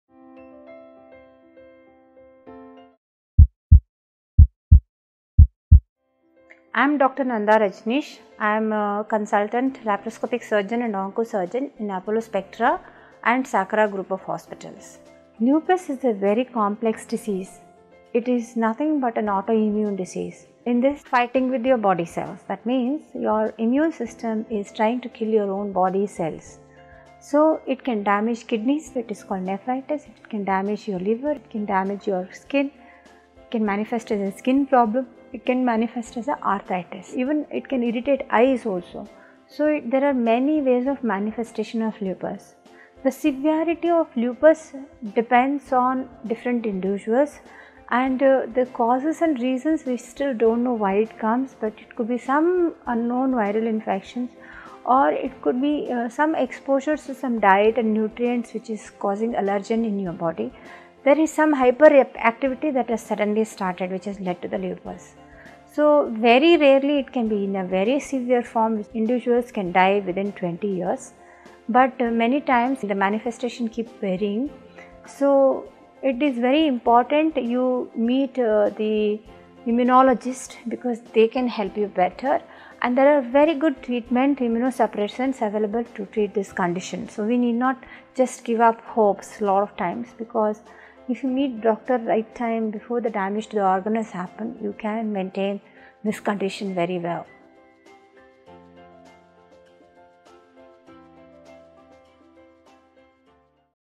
Lupus: Causes, Types & Flare- Ups. Can I live long with LUPUS?-Dr. Nanda Rajaneesh | Doctors' Circle
Dr. Nanda Rajaneesh | Appointment booking number: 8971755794
Consultant Onco Surgeon and Laparoscopic Surgeon|Apollo Spectra Hospitals, Bengaluru
Lupus is a very complex disease.  It is a nothing but an autoimmune disease. This means fighting with your body cells, your immune system is trying to kill your own body cells. So it  can  damage  kidneys, so it is  called as nephritis, it  can  damage liver, it  can  damage  your skin, it  can manifest  as  a  skin problem it  can manifest as  arthritis. So it   can irritate eyes also there are many manifestations of lupus. The  severity of  lupus depends on different individuals and the causes  and the reasons we  still don’t  know why it  comes  is  it is  because of  some  unknown viral infection  or  it  could be  some exposure  to diet and nutrient that is causing allergen in your body. There is some hyperactivity that has suddenly started that has  lead  to the lupus. So very rarely it can be in a very severe form and the individual can die in 20 years, but many times  the  manifestation keeps  varying.  So it is very important that you meet the immunologist, so they can help you better  and there are very  good treatment, immunosuppressants to treat these. So we  don’t need to give up hope  because if  you meet  a doctor at the right time before the  damage  to the organ has happened you can manage this condition very  well.